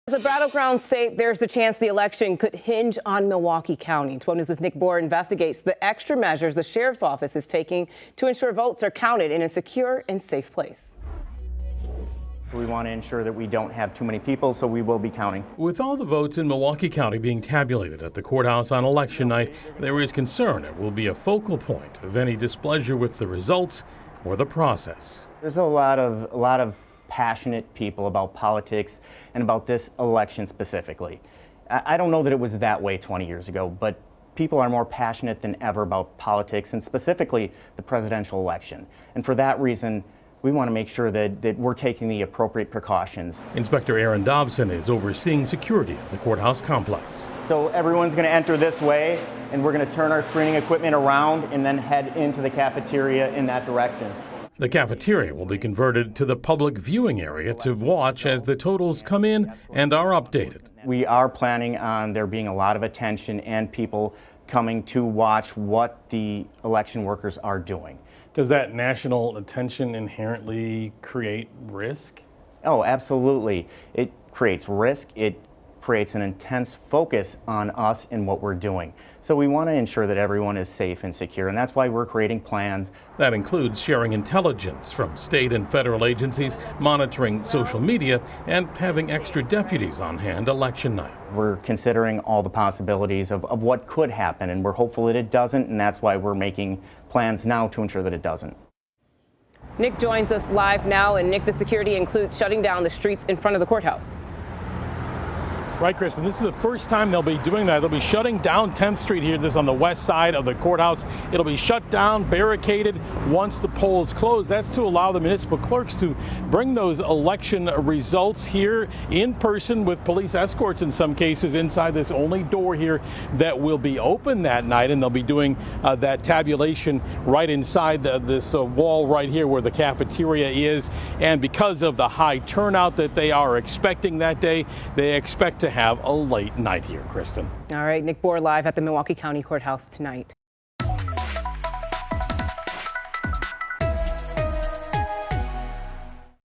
Election night preparations underway at Milwaukee County Courthouse
Staff is bracing for ballot scrutiny and attention on Wisconsin because it's a battleground state.